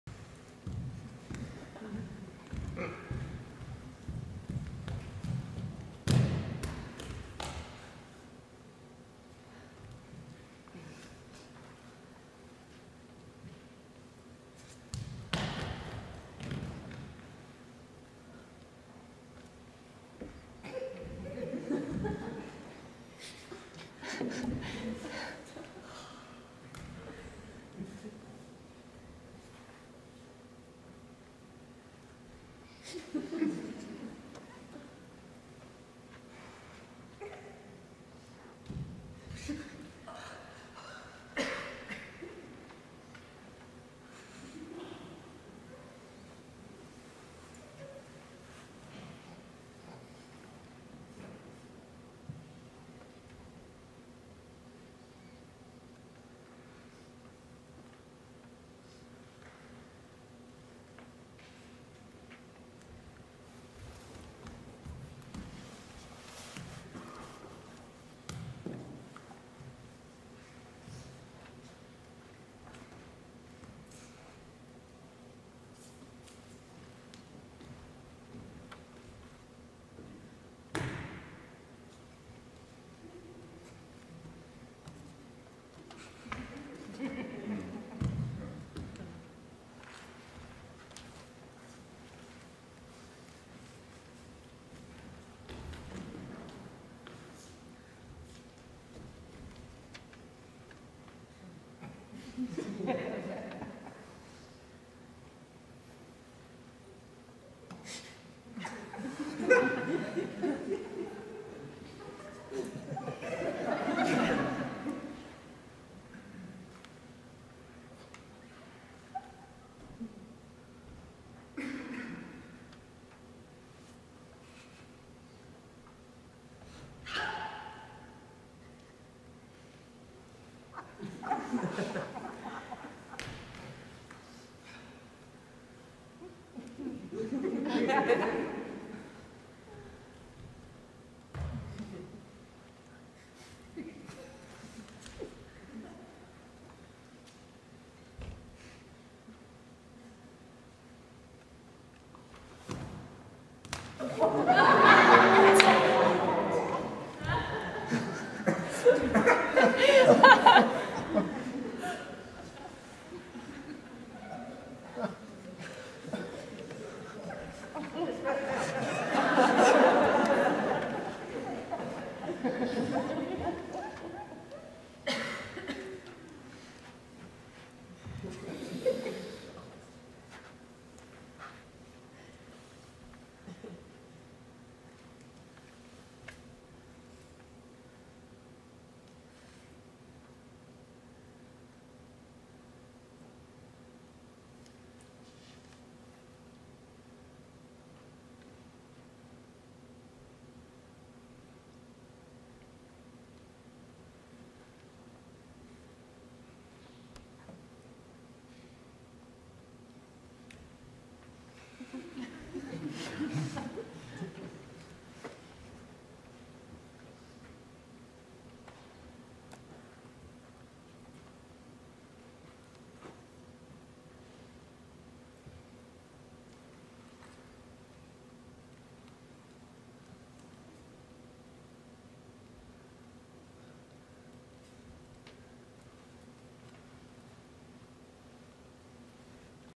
Jennifer Monson & Jennifer Miller - Movement Research at Judson Church March 14, 2016
Out of the Fray
Created and Performed by Jennifer Miller and Jennifer Monson
Videographer: David Gonsier